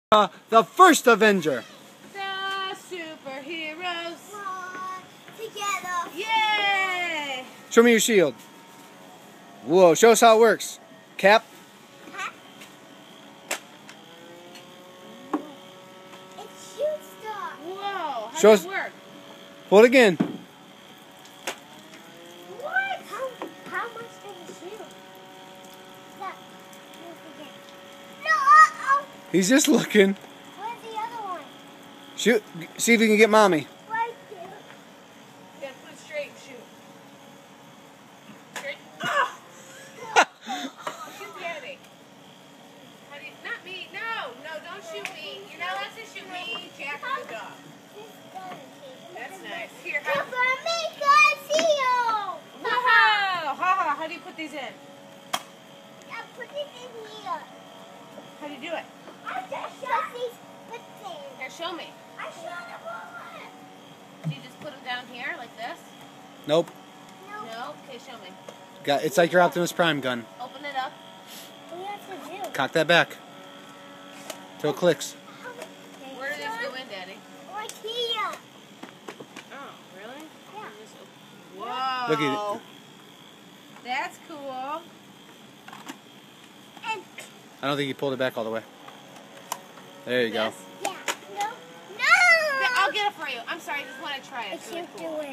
Finn's 3rd bday
Trying out his new toys.